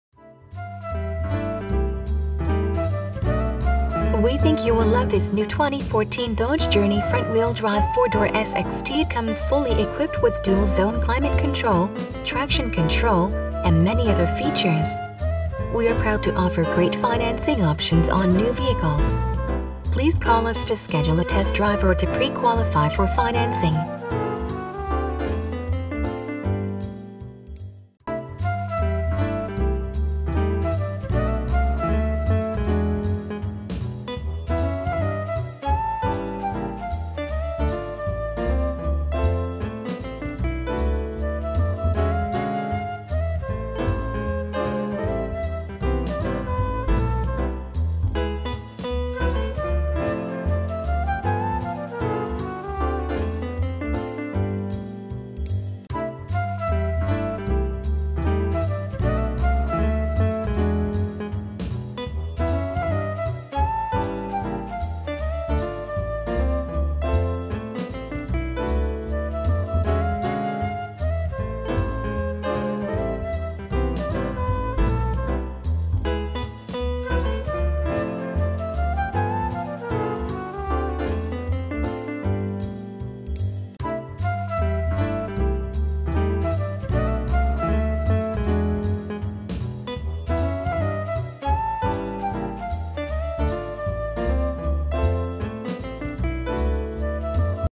2014 Dodge Journey FWD 4dr SXT
2014 Dodge Journey FWD 4 Door SXT
Stock Number: D4367
VIN: 3C4PDCBG6ET280595

We think you will love this NEW 2014 Dodge Journey FWD 4 Door SXT.It comes fully equipped with Dual Zone Climate Control, Traction Control, and many other features.We are proud to offer great financing options on new vehicles.